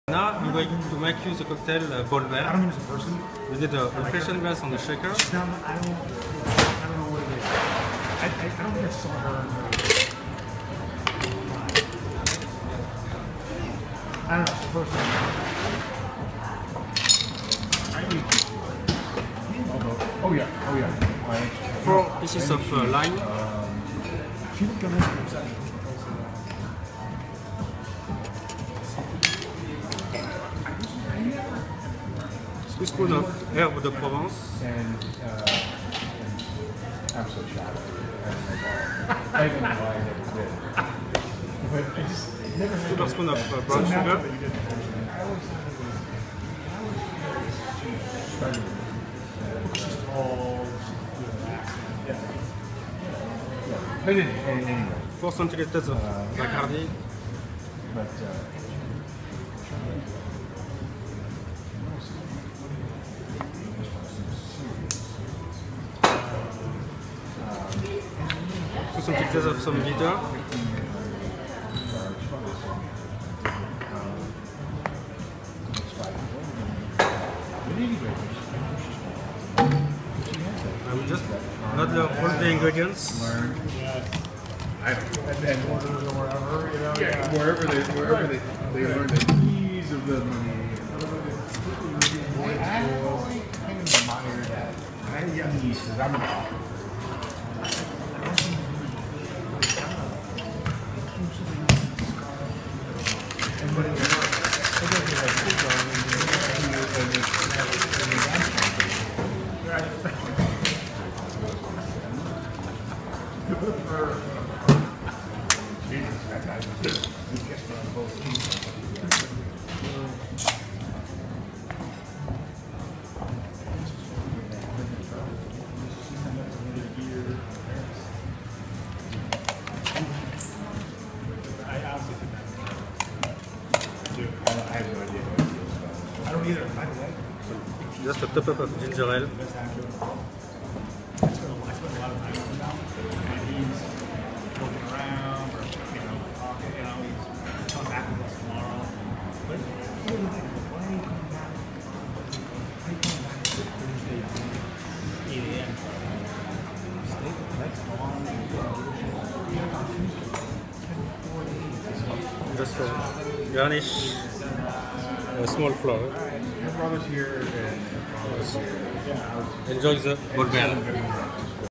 Mathieu Le Feuvrier From The Peninsula Hotel (Paris) : Bolber
Mathieu Le Feuvrier From The Peninsula Hotel in Paris makes for us his signature cocktail the Bolber